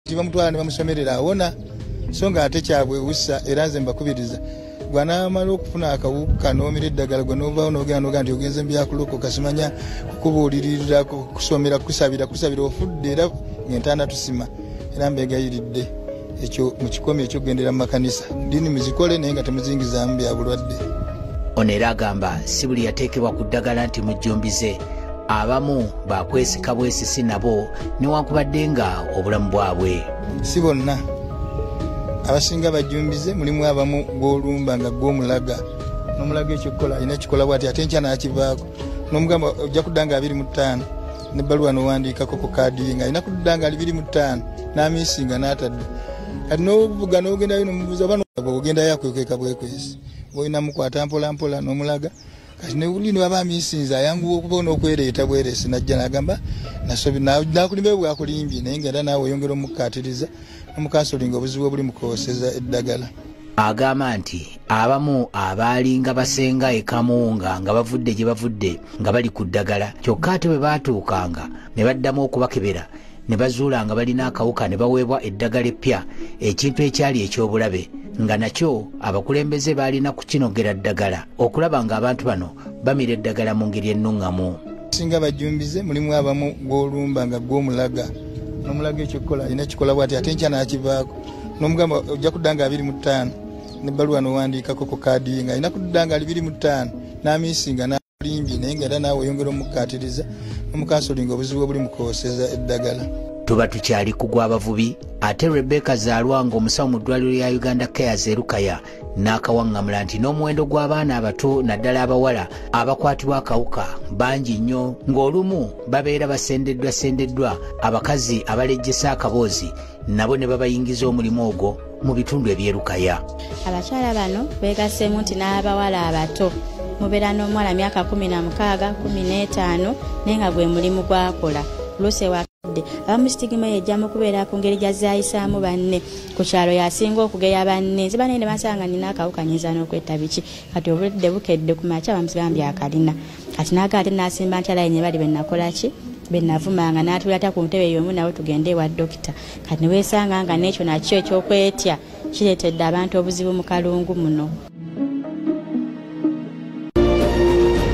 Emboozi ey’enjawulo ku kawukaTuli lukaya, eno abajjanjaba ab’akawuka beemulugunya
Emboozi ey’enjawulo ku kawuka
Tuli lukaya, eno abajjanjaba ab’akawuka beemulugunya, anti abantu abatya okunona eddagala bangi nnyo nnyo kyokka ate ng’abeetunda bangi abakwatiddwa obulwadde.